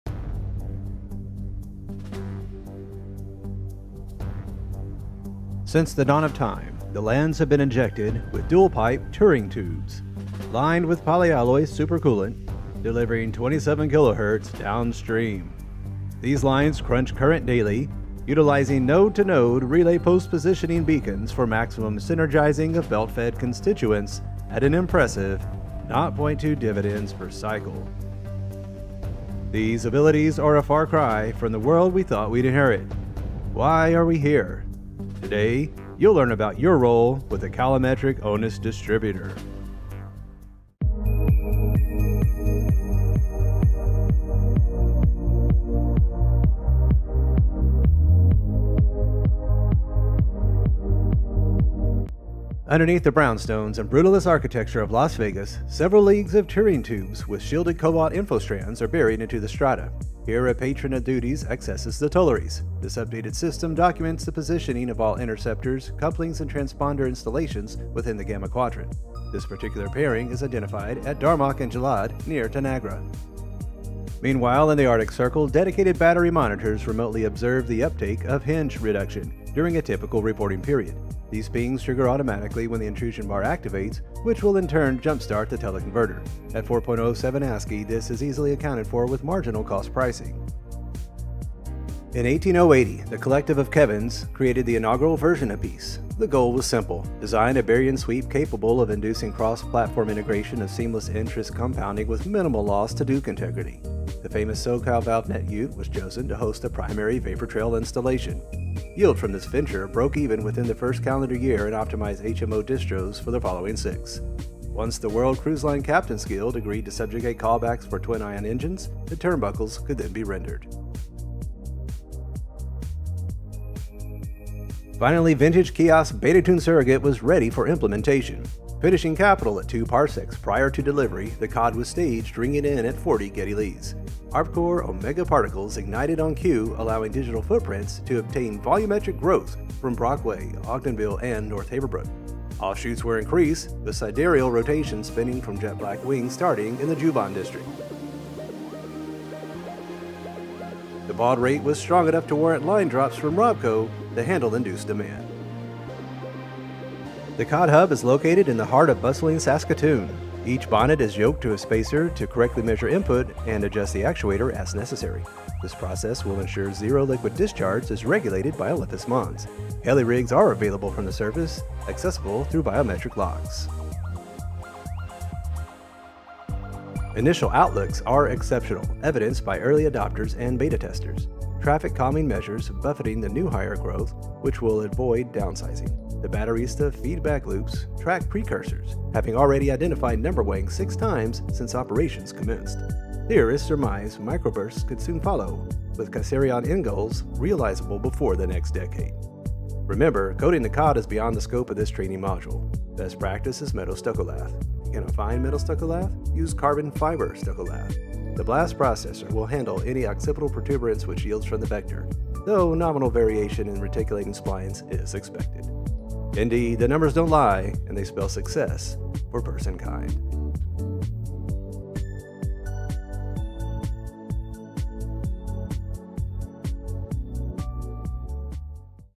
Understanding the Calimetric Onus Distributor
Since the dawn of time, the lands have been injected with dual-pipe Turing tubes, lined with poly alloy super coolant, delivering 27 kilohertz downstream. These lines crunch current daily, utilizing node to note relay positioning beacons for maximum synergizing of belt-fed constituents at an impressive nought point two dividends per cycle.

And then I wrote and recorded everything through a head cold while on Spring Break. EDIT: script is here if you're curious.